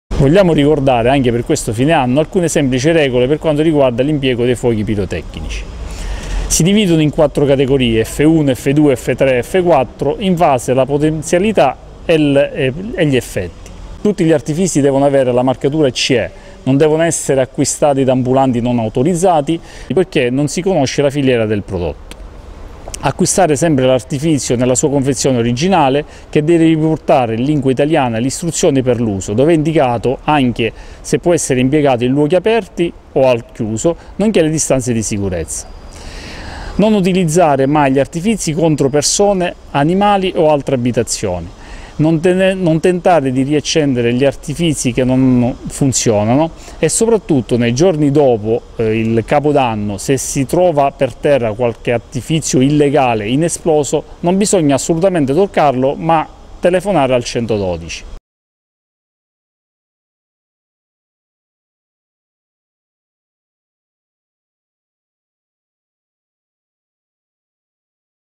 Carabinieri artificieri Catania, consigli su corretto utilizzo fuochi pirotecnici
Un veloce video inerente alcuni consigli forniti dal responsabile artificiere di questo C.do Provinciale, Lgt. C.S.  Salvatore Lizzio, sul corretto utilizzo dei fuochi pirotecnici in occasione dell’imminente Capodanno.
Notizie e approfondimenti di cronaca, politica, economia e sport con foto, immagini e video e molto altro ancora!